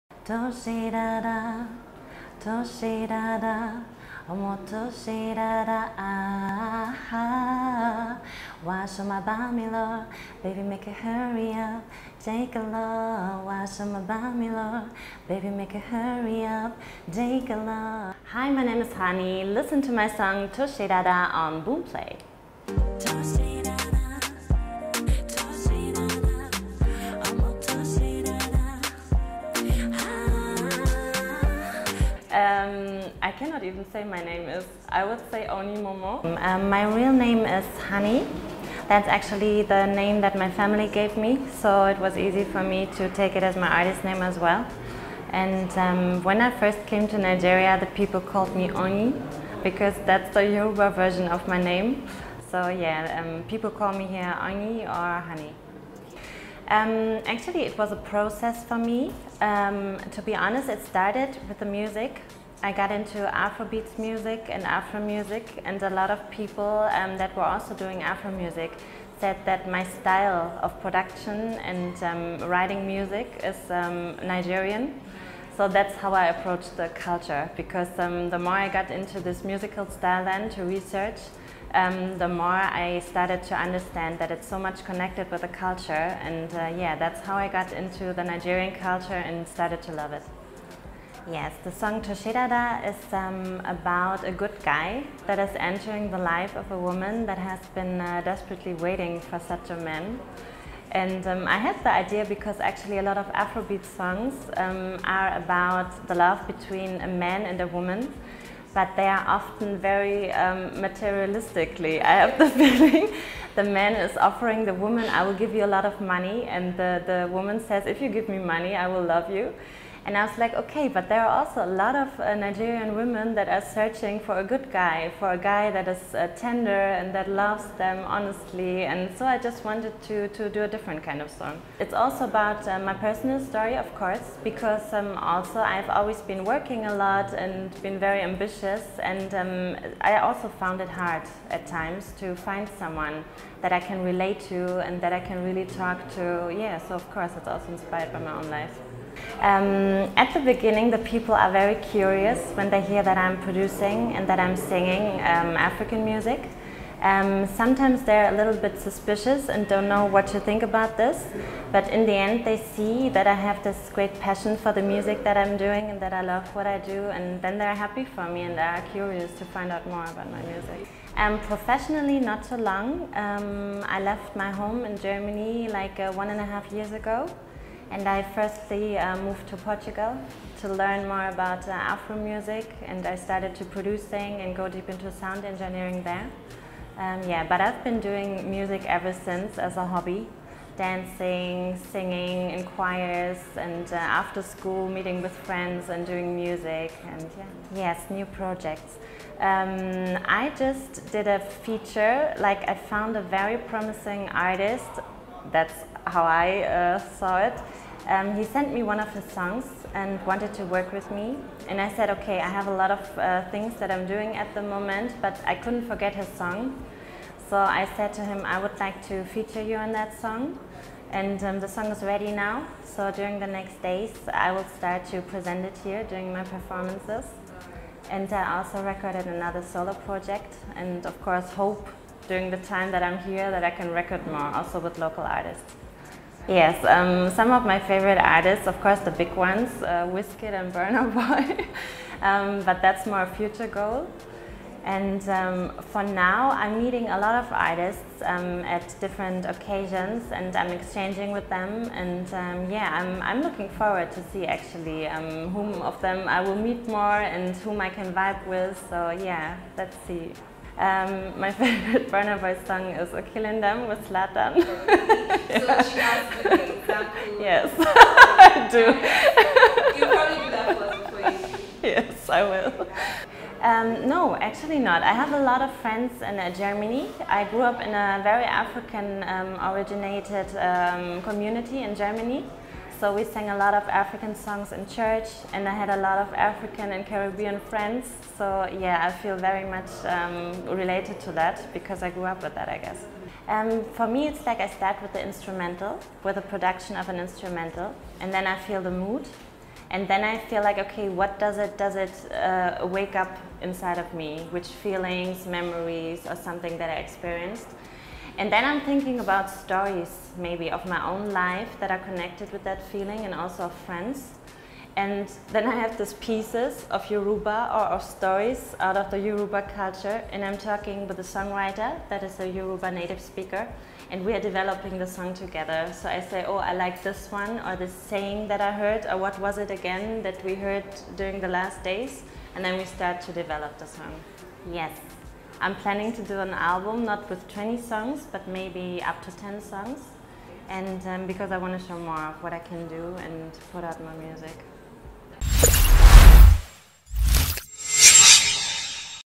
The Convo || Why I Love The Nigerian Culture- German Singer, Oyin.
Honey popularly known as Oyin Speaks on why she loves Afrobeats and chose to sing in Yoruba despite the fact that she is German.